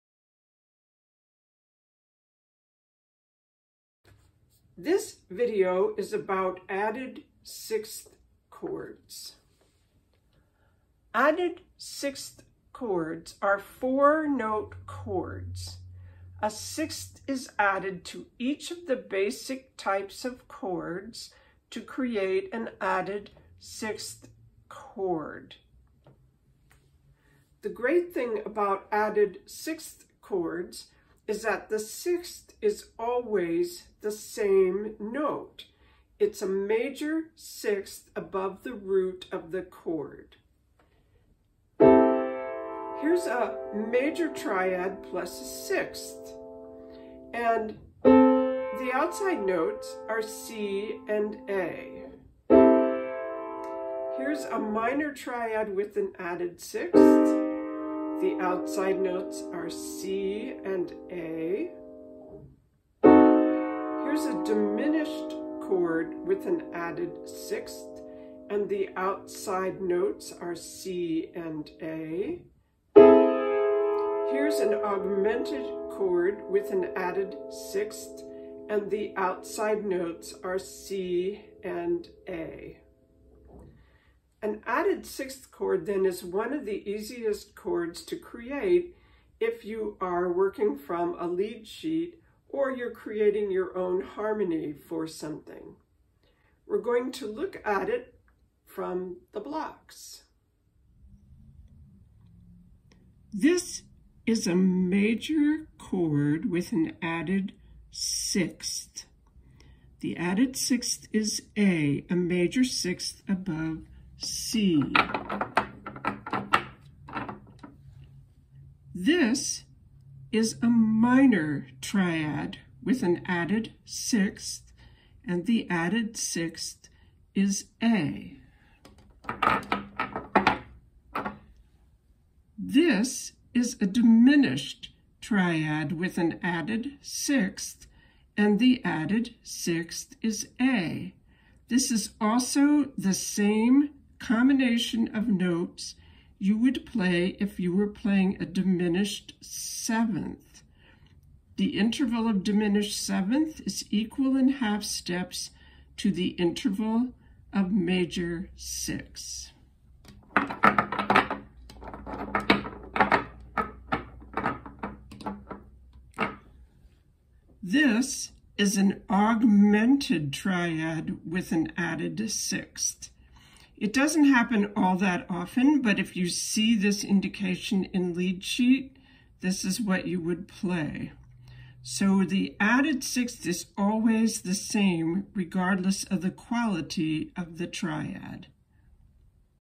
Added Sixth Chords
How added sixth chords are structured and what they sound like.